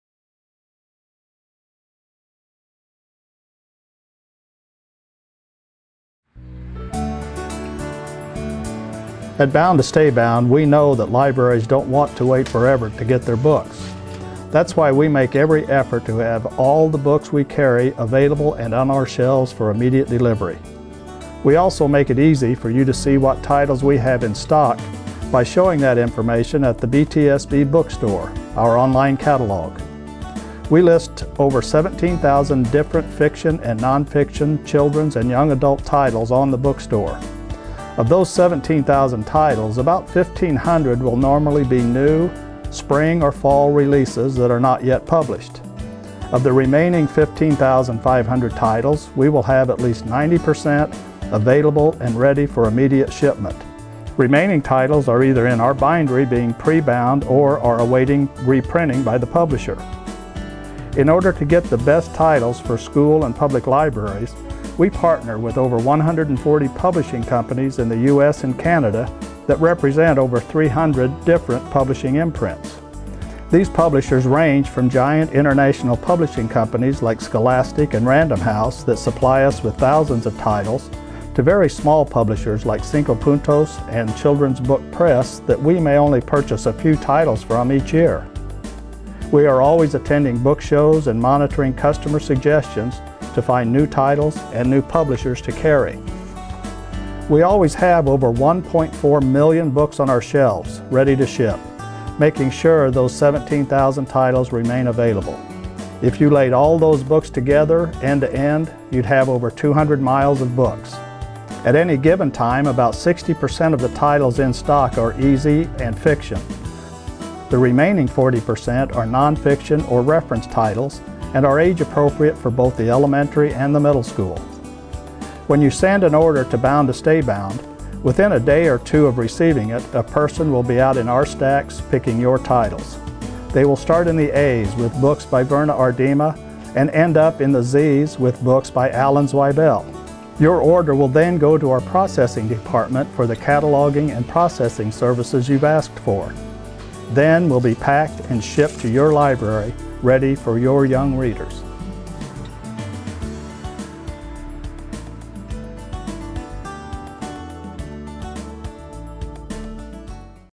200 Miles of Books: A Tour of Bound to Stay Bound Books
Mike Brady takes us on a tour of the Bound to Stay Bound inventory showing us how Bound to Stay Bound can deliver our products to your library soon after an order has been made.